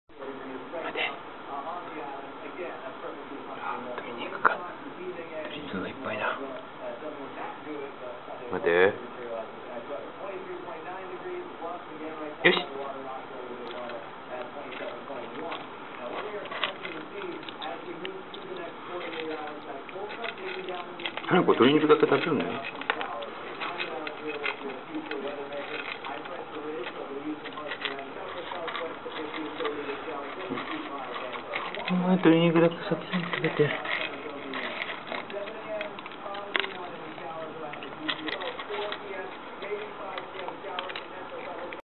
20080801 好きなおかずは・・・・
好きなご馳走から食べるやつ

鶏肉混ぜご飯を食べるハナ子